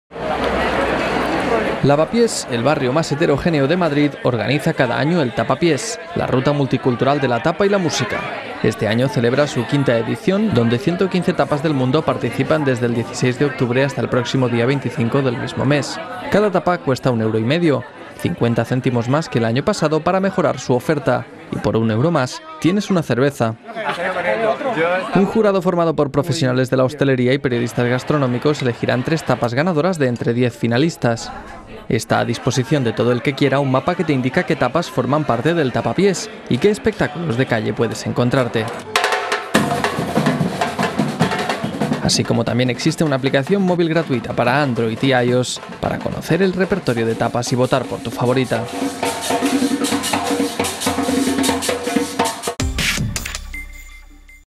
Tapapiés: Reportaje de Zoomin TV 19 10 15
Tapapiés: recorremos Lavapiés de tapa en tapa